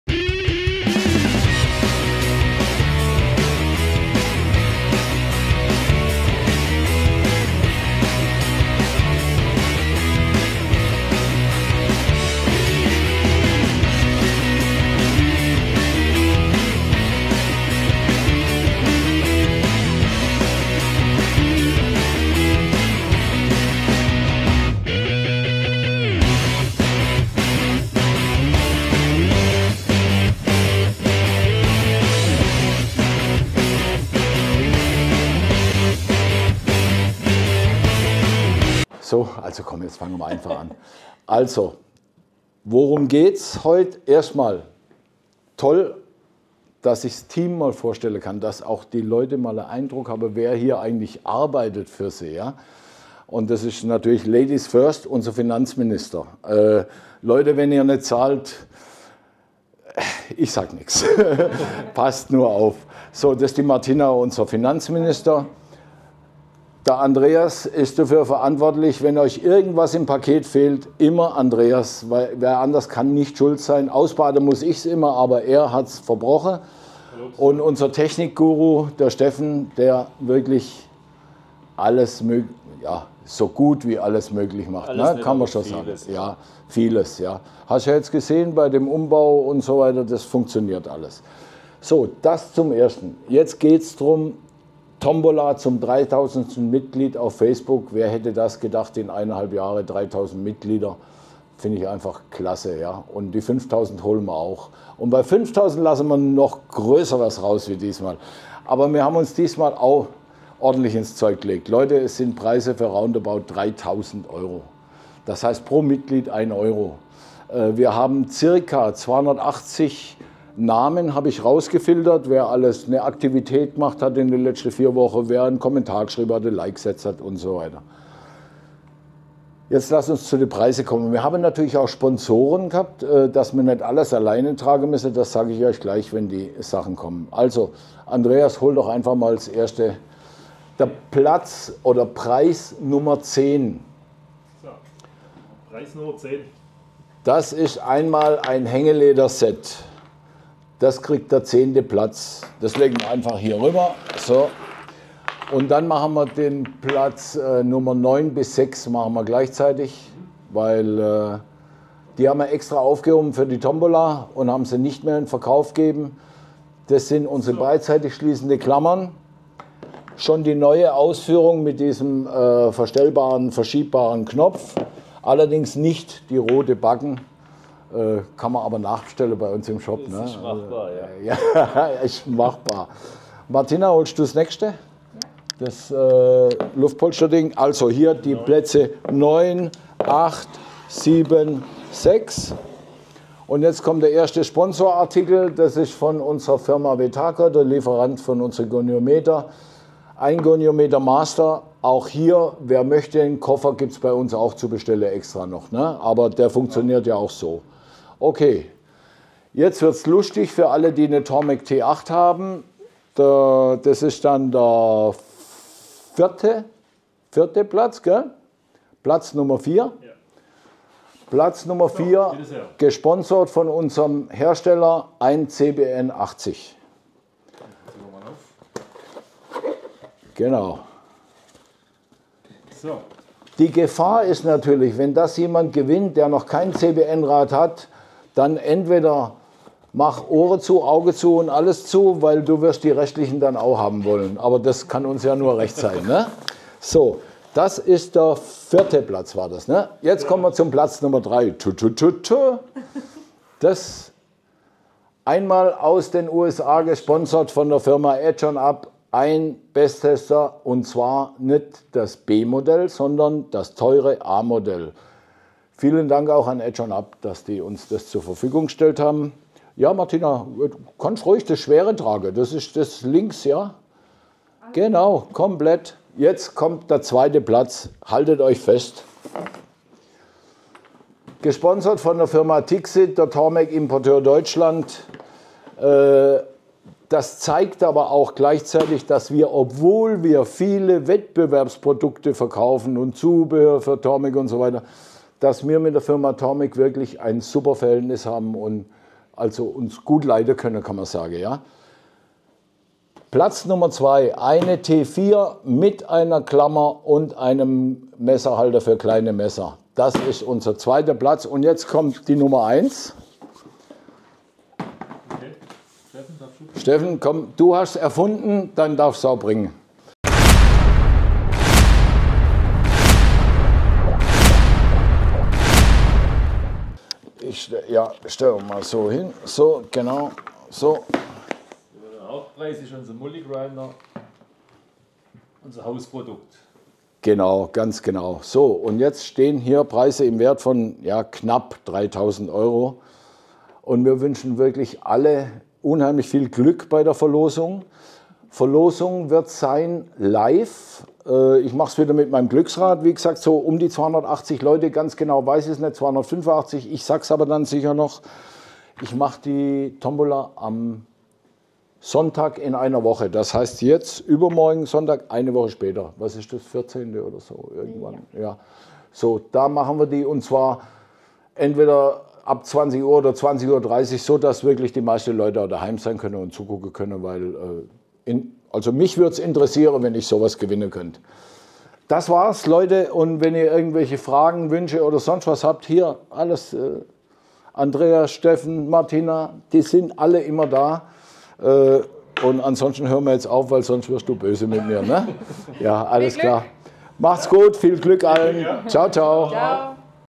Vorstellung unserer Tombola Preise
teilnahmeberechtigt waren alle Mitglieder unserer Facebook Gruppe "SchleifJunkies" welche in den letzten 30 Tagen in irgendeiner Form in der Gruppe aktiv waren. Vielen Dank an euch alle. Mit und von euch lebt die Gruppe. Wir können glaube ich stolz sein was wir in solch kurzer Zeit auf die Beine gestellt haben. Dazu tragen selbstverständlich die Mitglieder der Gruppe maßgeblich bei. Es ist ein hohes Schwarmwissen, für jeden der sich für schleifen von Messern und anderen Tools interessiert, vorhanden.

Diesen Erfolg der Gruppe gilt es natürlich zu feiern.
Das wollen wir gemeinsam mit euch machen.
Wir bedanken uns ganz herzlich bei unseren Sponsoren die so großzügig Unterstützung geleistet haben um euch die Möglichkeit zu bieten einen tollen Preis in unserer Tombola zu gewinnen.

Die Live Auslosung wird voraussichtlich am 14.04 so gegen 20-20:30 Uhr sein. Genaues kommt in der Gruppe noch.